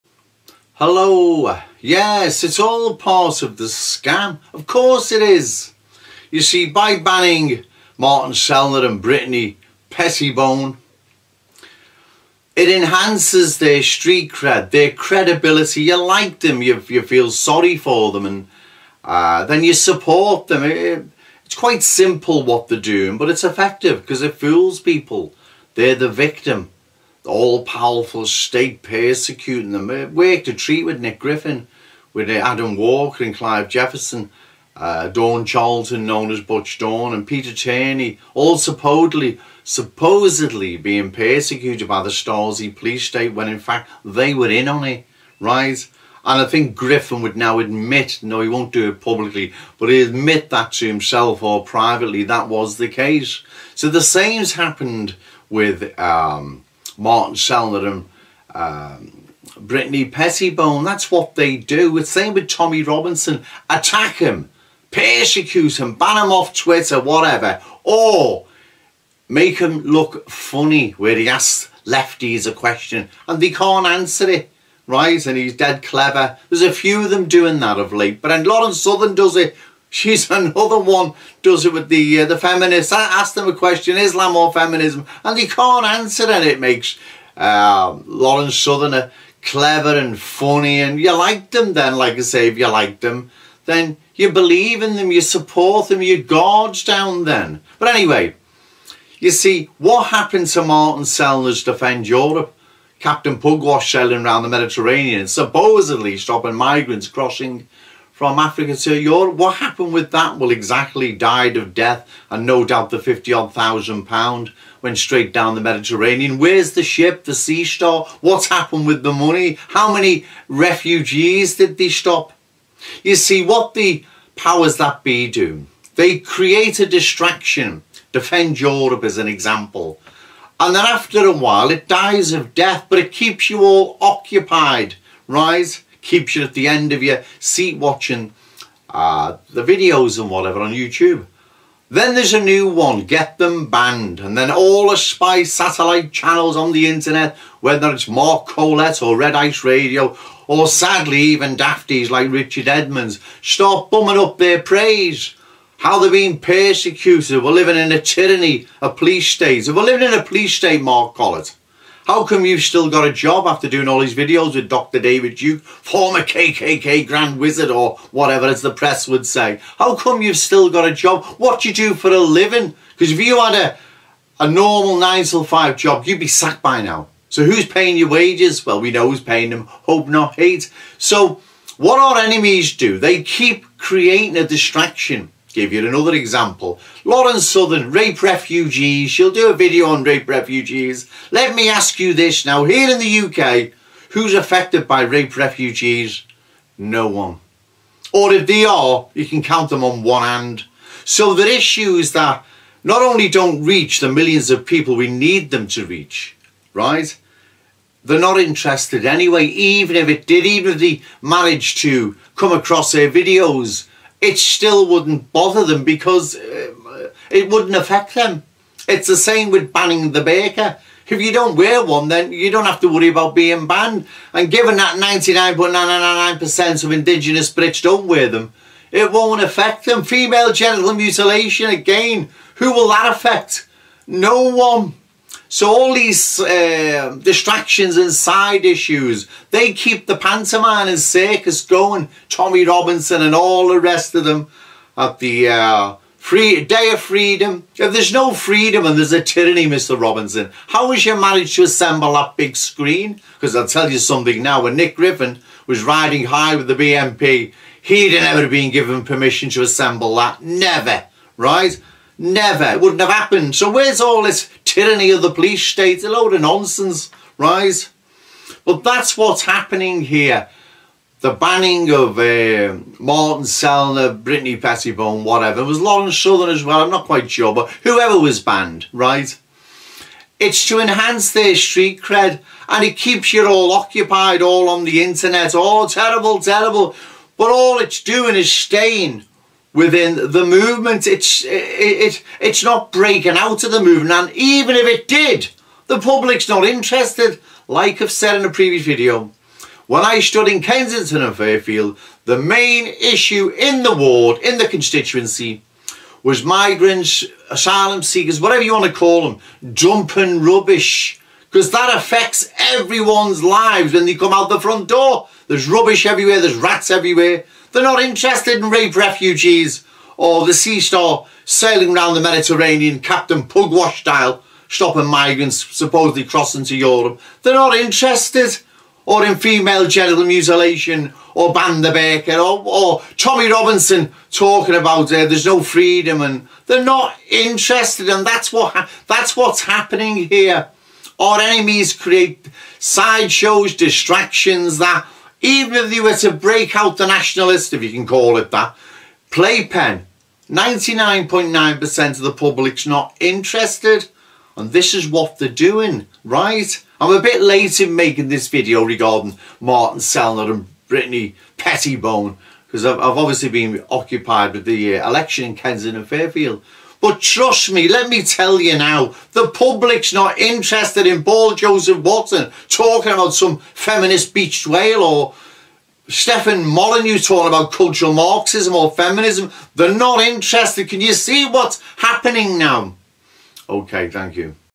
Martin Sellner and Brittany Pettibone banning is ALL part of the SCAM!
Martin Sellner and Brittany Pettibone are Genuine Racial Nationalists, but agents of those out to STOP US!